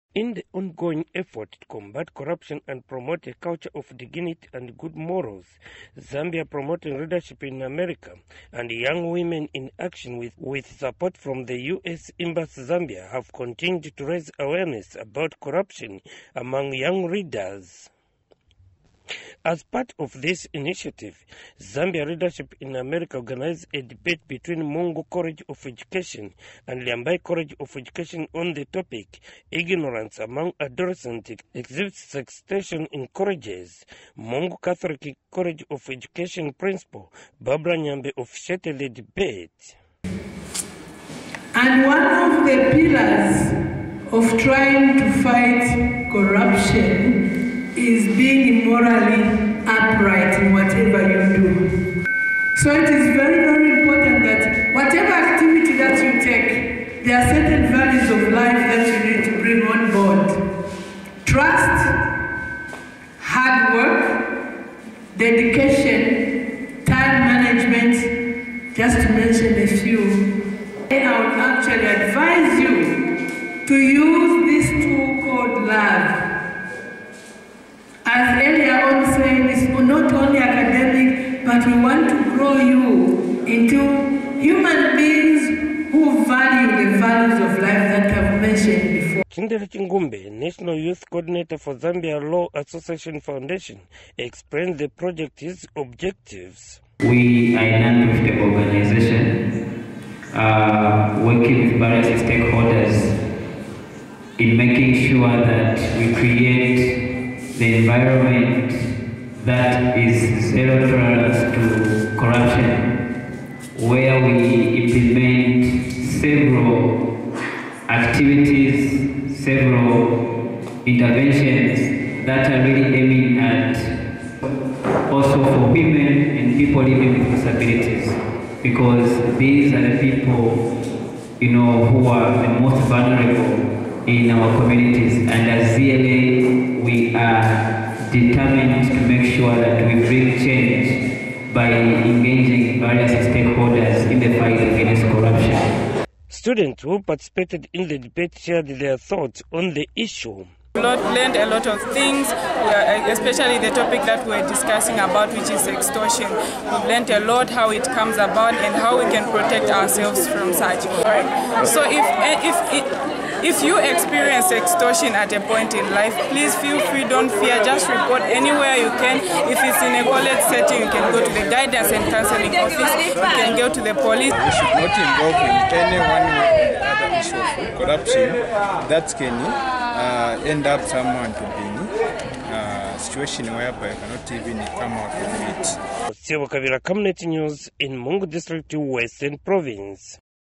ZLA ARIPICK Third Debate in Mongu
In the continued quest to combat corruption and create cultures of integrity  Zambians Promoting Leadership in America - ZLA Foundation, in Collaboration with Young Woman in Action - YWA, with financial support from the US Embassy Zambia, have continued to create awareness on corruption through colleges debates.

On 28th July, ZLA conducted a college debate under the motion " *IGNORANCE AMONG STUDENTS EXACERBATES SEXTORTION IN COLLEGES OF EDUCATION"* .  190 participants attended this lively discussion that was held in Mulambwa ward.

The event was graced by the Mongu Catholic College of Education Principal..

 The two education colleges were
1. Mongu Catholic College of Education
2. Lyambai Teachers College of Education

 *The Judges Were from:*
1. Ministry of Education
2. Civil Society Organization ( ZYL)

 *Take Away:*
1. Many Youth didn't know that sextortion contributes to the existence of corruption in the education sector.

 *The event was impactful in a manner that, after the debate session, debaters from both institutions and attendees engaged into a discussion to further their understanding on sextortion in education colleges as it relates to corruption.*

The event was televised by a media house, CAMNET TV.